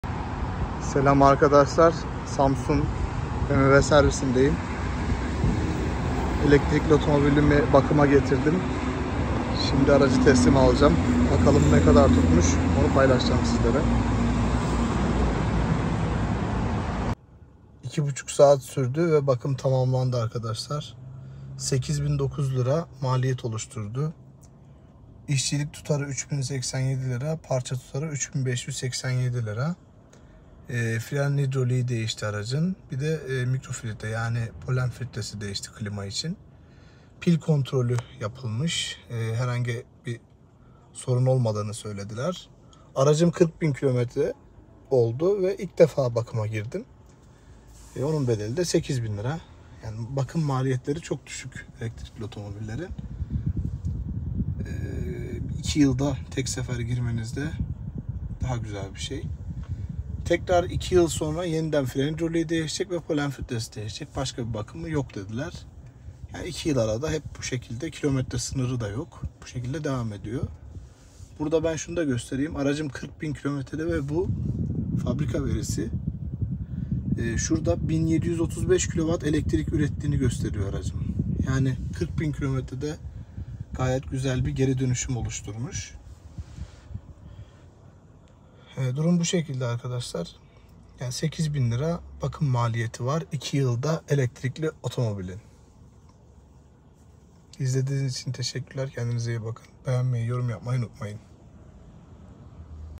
Elektrikli Bmw bakım ücreti, elektrikli otomobil servis bakımı ne kadar, Elektrikli araç bakımı
Elektrikli otomobilim 2. Yıl sonunda 40 bin km de ilk servis bakımını paylaştığım içeriktir, iyi seyirler dilerim.